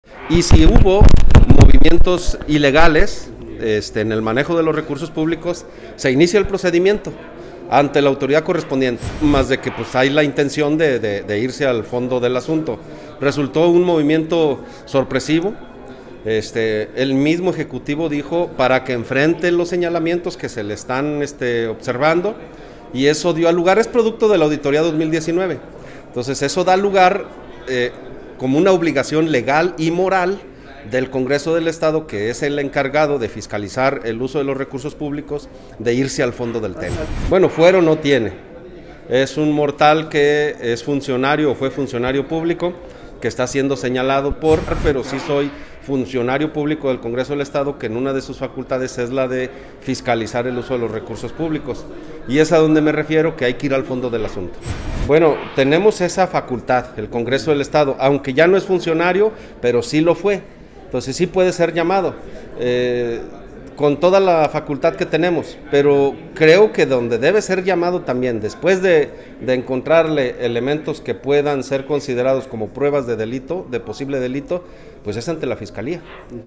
Jorge López, un mortal sin fuero que debe ser llamado a cuentas por la Fiscalía
Tras recordar que el expresidente del Patronato de la Feria Nacional de San Marcos, Jorge López Martín es un “mortal sin fuero”, el diputado local por el Partido de la Revolución Democrática (PRD), Cuauhtémoc Escobedo Tejada, advirtió que los presuntos desvíos cometido por el funcionario no serán echados al olvido y, por el contrario, se irá en su contra hasta las últimas consecuencias.
En entrevista, el legislador reiteró que, de comprobarse las responsabilidades derivadas de los malos manejos encontrados durante la revisión del ejercicio 2019 en la Secretaría de Turismo y el propio Patronato de la Feria, se dará parte a la autoridad correspondiente, incluso la Fiscalía Anticorrupción.
Recordó que si bien el Congreso del Estado tiene la facultad de llamar a comparecer a López Martín y no se descarta exigirle se presente ante los diputados, de donde también deberá llamársele a cuentas es ante la Fiscalía General del Estado.